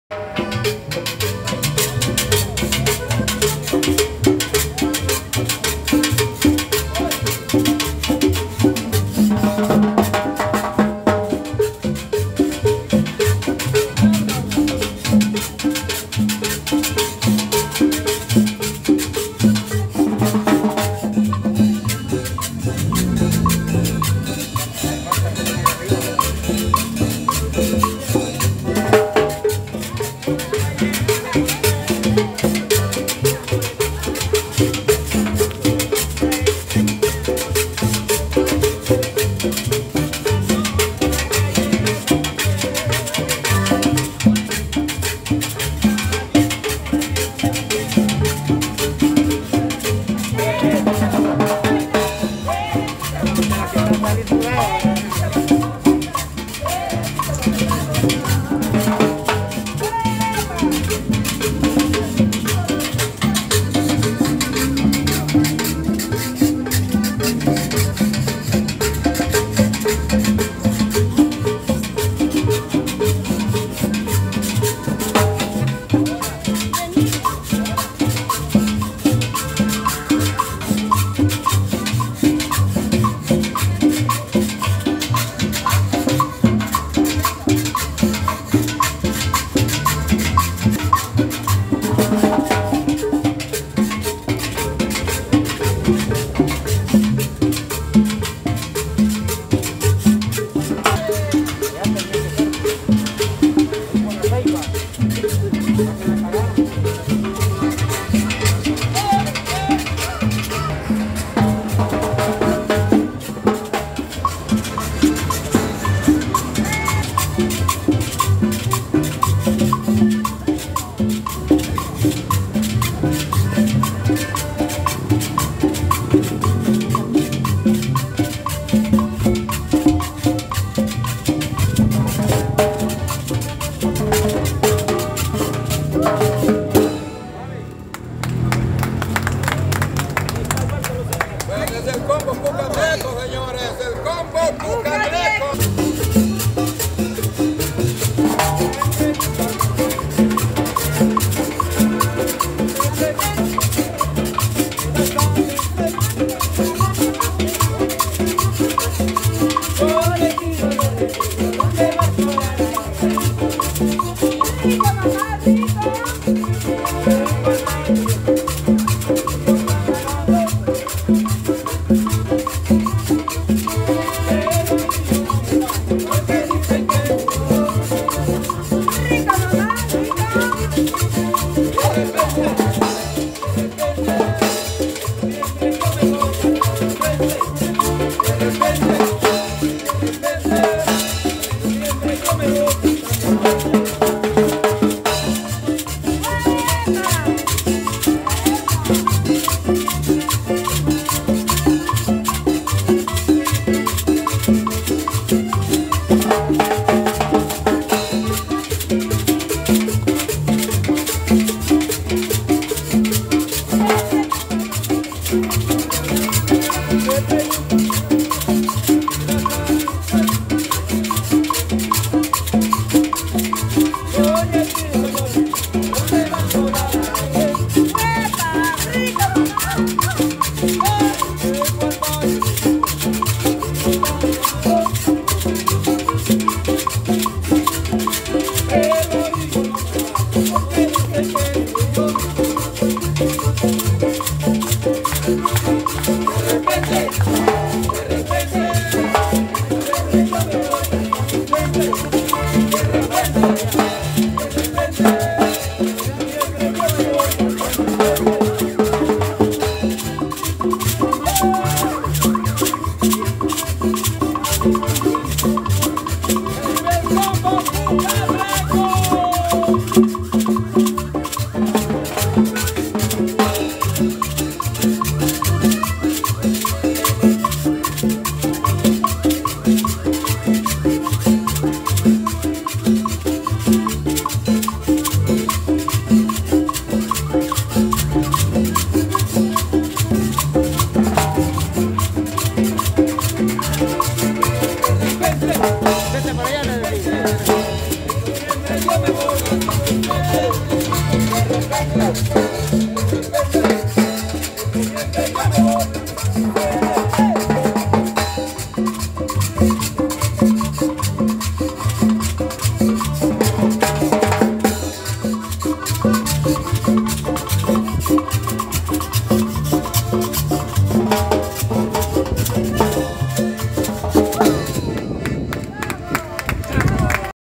KEYRI vr CELINA QUIEN BAILA MEJOR DE ELLAS
TODOS BAILAN EN LA PLAZA ZURITA DE SAN SALVADOR